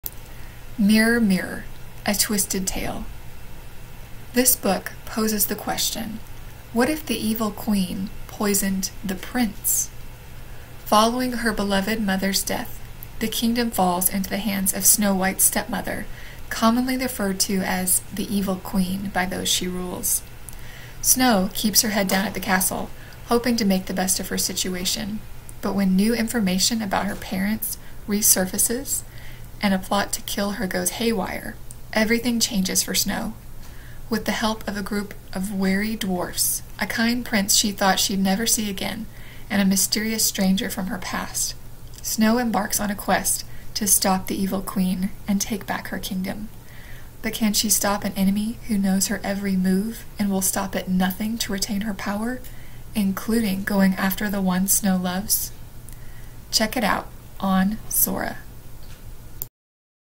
Mirror, Mirror : A Twisted Tale by Jen Calonita
Book Talk
Mirror, Mirror: A Twisted Tale poses the question, what if the Evil Queen poisoned the prince?

Following her beloved mother's death, the kingdom falls into the hands of Snow White's stepmother, commonly referred to as "the Evil Queen" by those she rules. Snow keeps her head down at the castle, hoping to make the best of her situation.

But when new information about her parents resurfaces and a plot to kill her goes haywire, everything changes for Snow. With the help of a group of wary dwarfs, a kind prince she thought she'd never see again, and a mysterious stranger from her past, Snow embarks on a quest to stop the Evil Queen and take back her kingdom. But can she stop an enemy who knows her every move and will stop at nothing to retain her power... including going after the ones Snow loves?Age Range: Teen